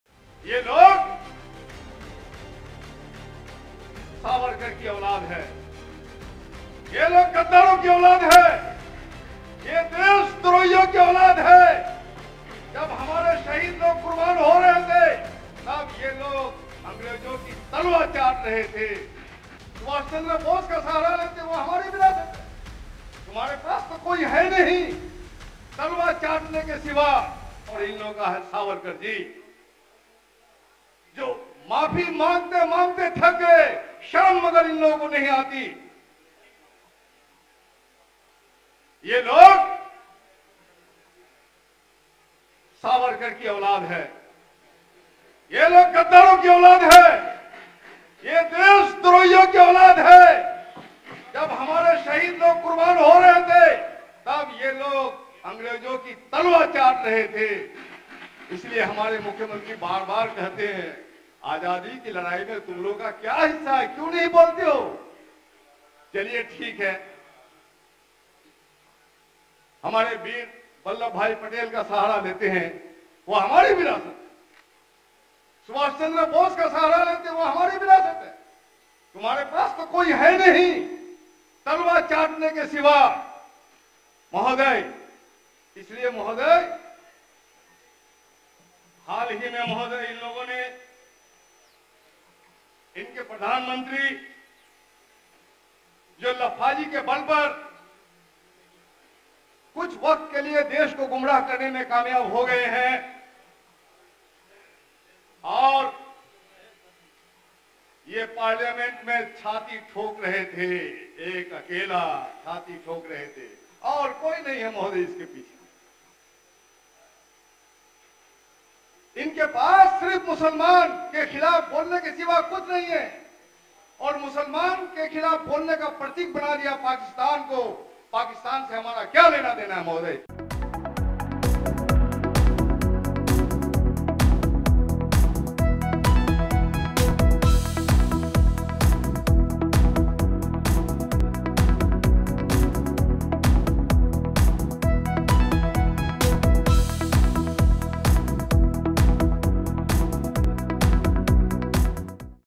Mehboob Alam ने BJP विधायकों को सदन में कहा सावरकर की औलाद, मच गया बवाल | Bihar Tak
माले विधायक महबूब आलम ने Bihar Vidhasabha में आपत्तिजनक बयान दिया. और बीजेपी पर जमकर हमला बोला है.